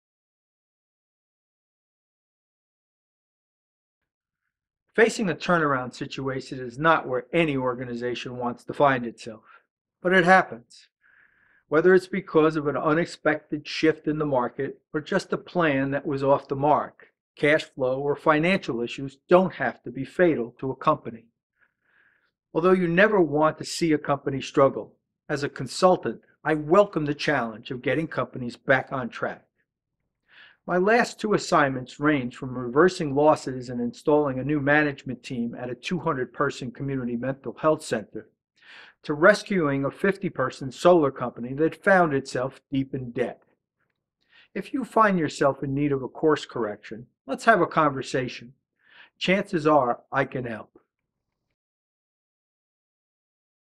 Company Turn Arounds
No company wants to find itself in a turn-around situation. But if you do, watch this video to see how I can help.